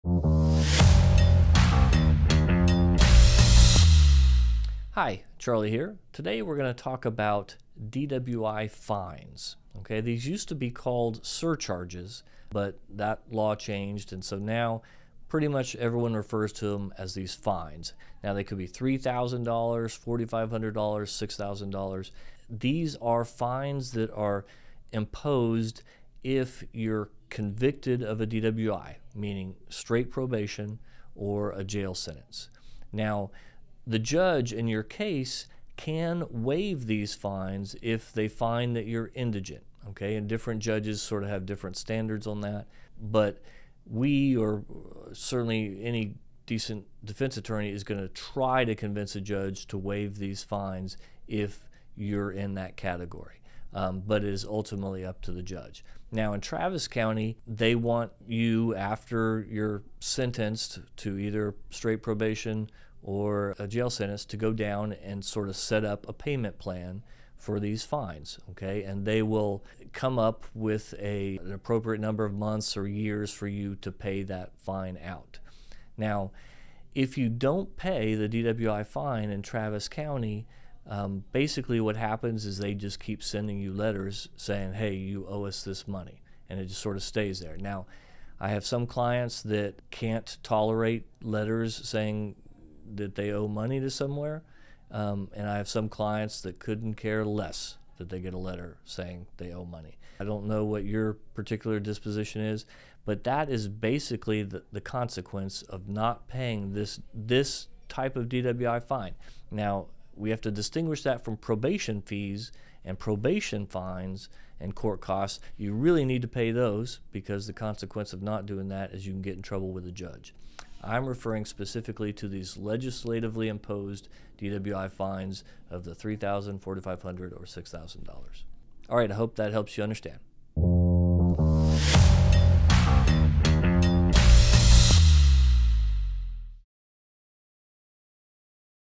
Understanding DWI Conviction Fines In Texas | Austin Criminal Defense
Austin Criminal Defense Attorney Charlie Roadman discusses DWI fines in Texas, previously termed surcharges. These fines, ranging from $3,000 to $6,000, are imposed upon conviction, applicable to both probation and jail sentences. Judges have the discretion to waive these fines for indigent individuals. In Travis County, individuals are expected to establish a payment plan for these fines. Non-payment results in continuous reminders but no severe consequences, unlike probation fees and court costs, which must be paid to avoid legal issues. Always ask your attorney about the consequences  in your jurisdiction

Charlie is the author of The Defendant's Guide to Defense: How to Help Your lawyer Get the Best Result (Misdemeanor Edition)